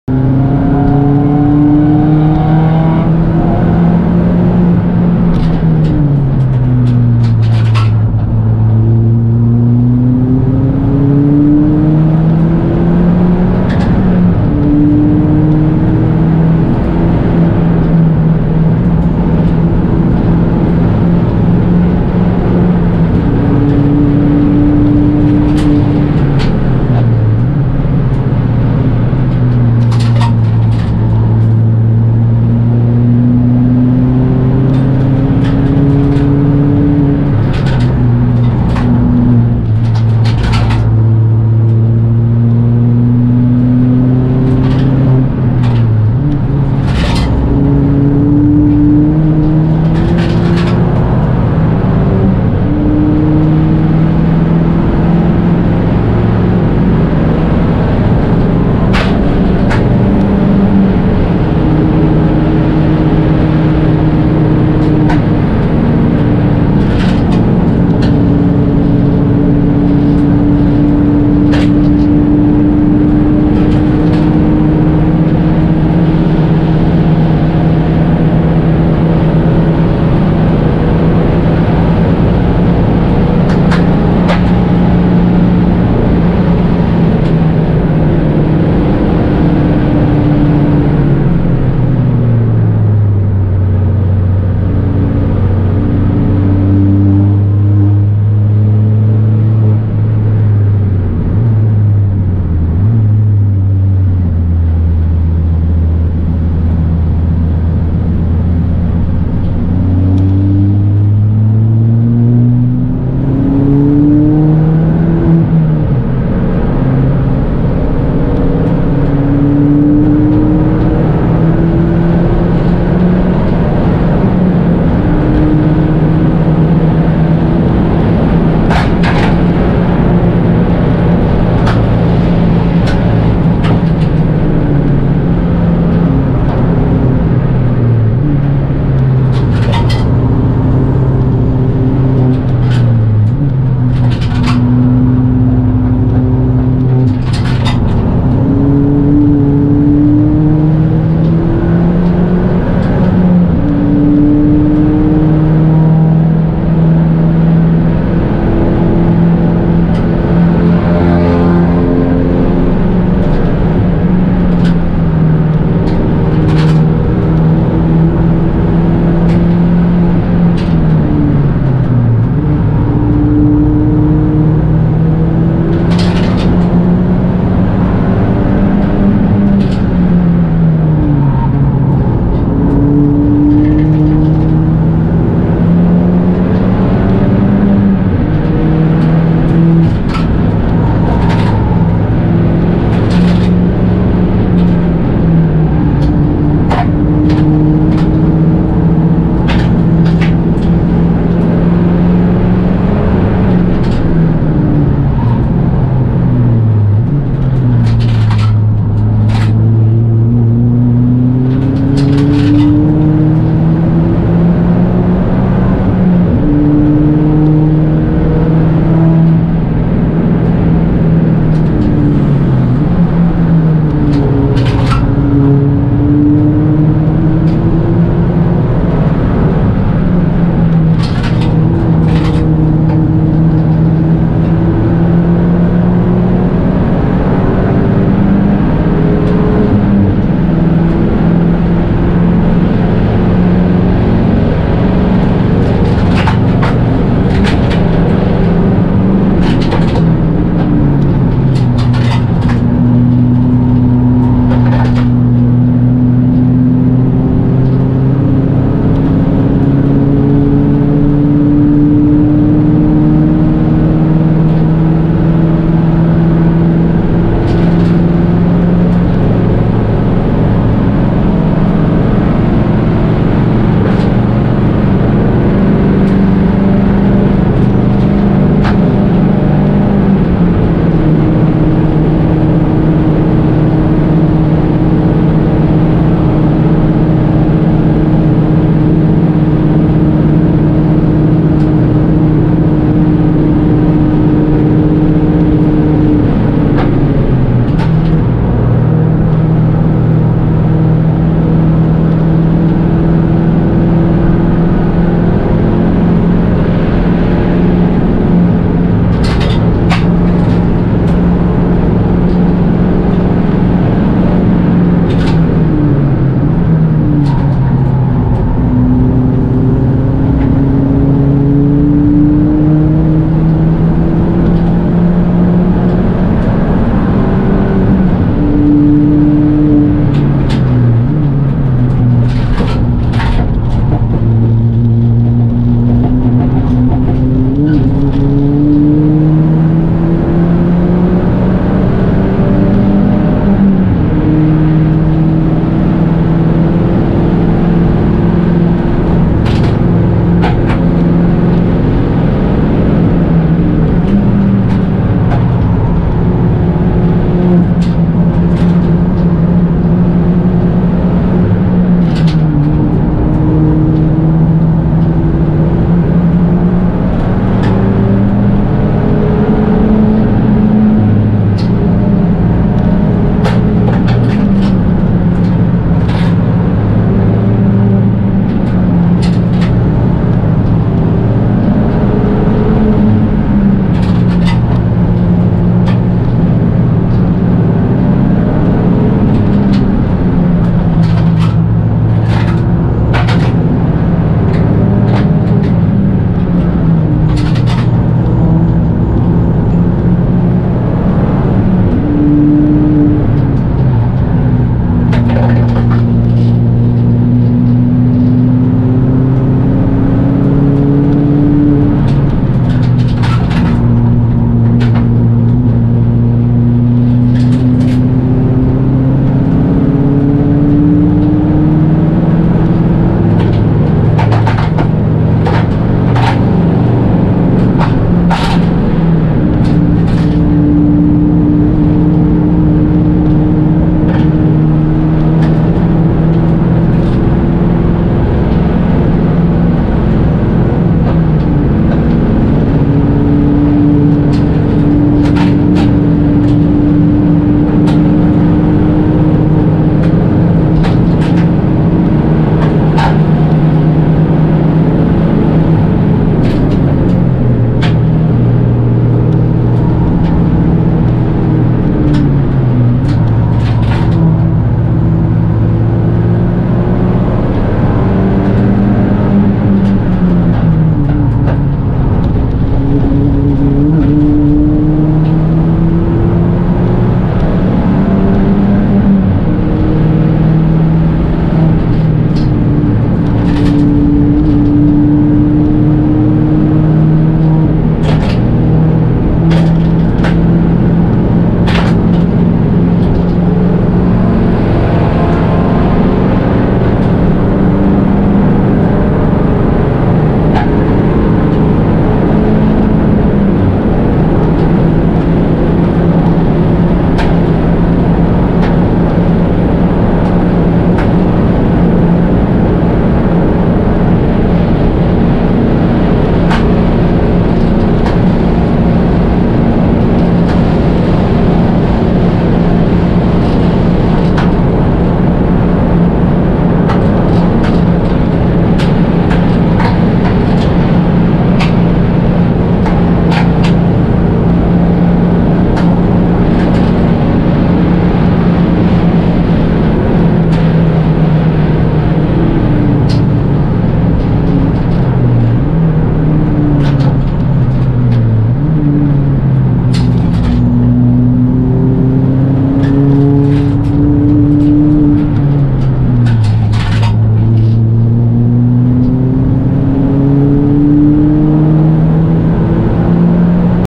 RCN 5 2021 | Buttler-Pal Motorsport | Ford Fiesta ST | 9:19 with code 60 and stuff
Follow me a lap around the nürburgring nordschleife with the Buttler-Pal Motorsport Ford Fiesta ST.